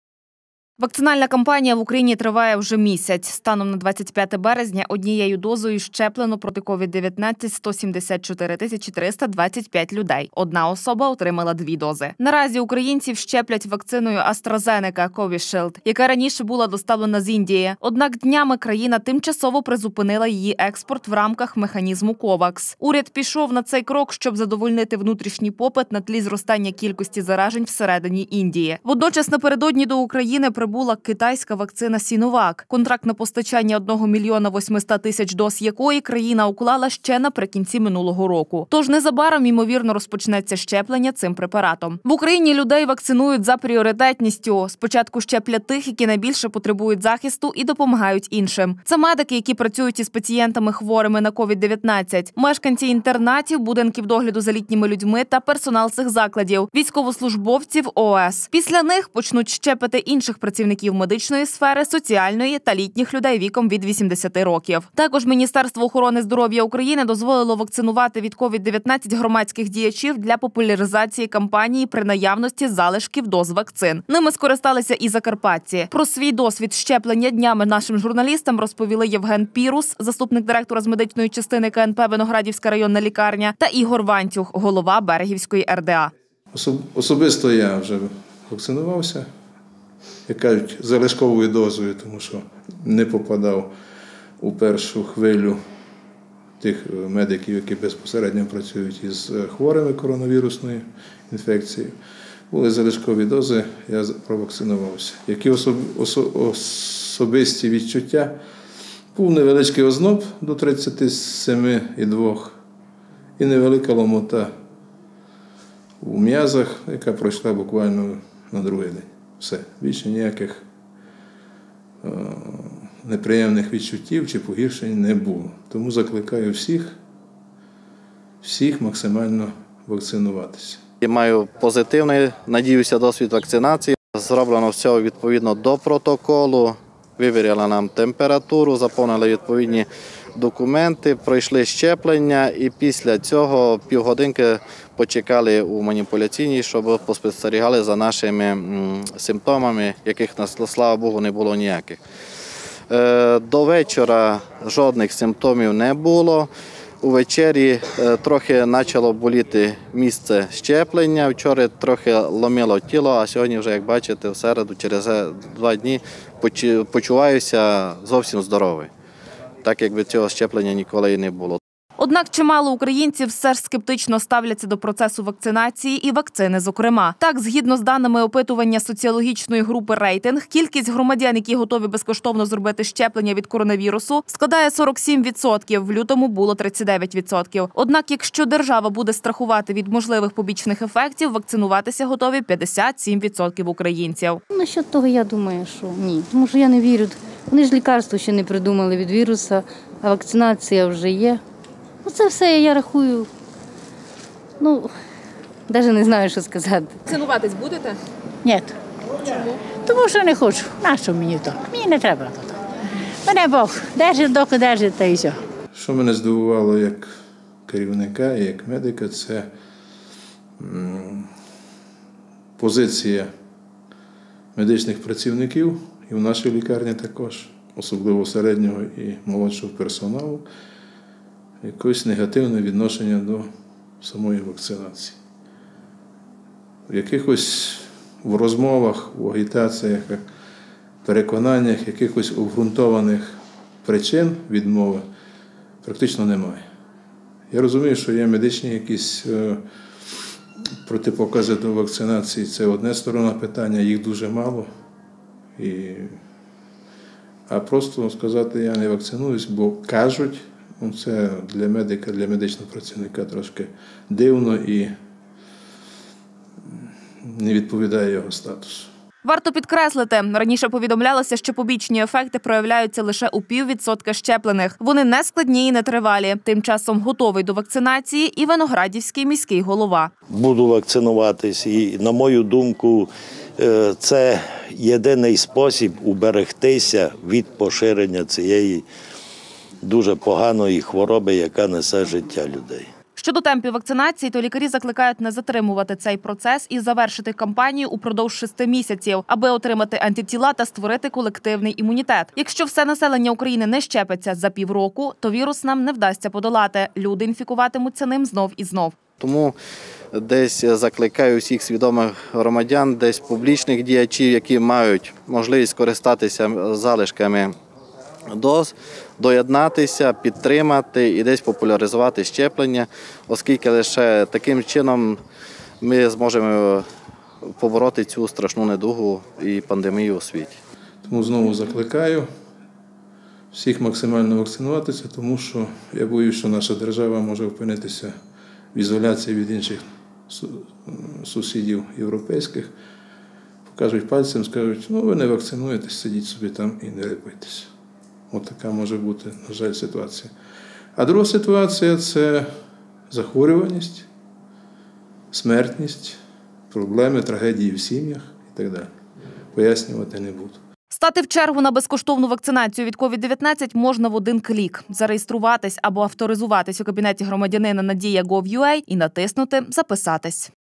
Старт вакцинації . Єдиний вихід?
24 лютого 2021 року в Україні зробили перше щеплення від COVID-19. Першу дозу вакцини AstraZeneca (Covishield) отримав черкаський  лікар-реаніматолог. Наразі ж на календарі кінець березня і одну дозу вакцини отримали 174 324 українці з понад 41 мільйона населення. На Закарпатті ж проведено усього 510 щеплень.
Вакцинальна кампанія в Україні триває уже місяць. Станом на 25 березня однією дозою щеплено проти COVID-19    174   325 людей, 1 особа отримала 2 дози.  Наразі українців щеплять вакциною  AstraZeneca (CoviShield), яка раніше була доставлена з Індії. Однак днями країна тимчасово призупинила її експорт в рамках механізму COVAX . Уряд пішов на цей крок, щоб задовольнити внутрішній попит на тлі зростання кількості заражень всередині Індії. Водночас напередодні до України прибула китайська вакцина Sinovac, контракт на постачання 1,8 мільйона доз якої країна уклала ще наприкінці минулого року. Тож незабаром імовірно розпочнеться щеплення цим препаратом.
В Україні людей вакцинують за пріоритетністю: спочатку щеплять тих людей, які найбільше потребують захисту і допомагають іншим. Це медики, які працюють із пацієнтами з COVID-19, мешканці інтернатів, будинків догляду за літніми людьми та персонал цих закладів, військовослужбовці в ООС. Після них почнуть щепити інших працівників медичної сфери, соціальної та літніх людей віком від 80 років.